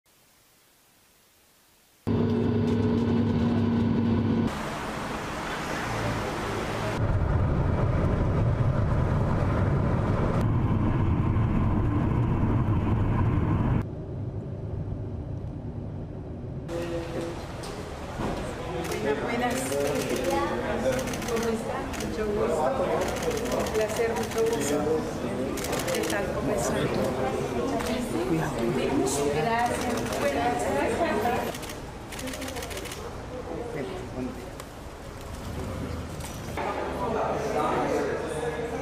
Presidenta Xiomara Castro llega a Brasil para investidura de Lula da Silva
NACIONALES🚨Así fue el arribo de la Presidenta Xiomara Castro a Brasil, donde participará en la toma de posesión de Lula da Silva el próximo 1 de enero de 2023. "Después de dos conexiones y más de 10 horas de vuelo hemos llegado a Brasília", comentó el secretario privado de la presidenta, Héctor Manuel Zelaya.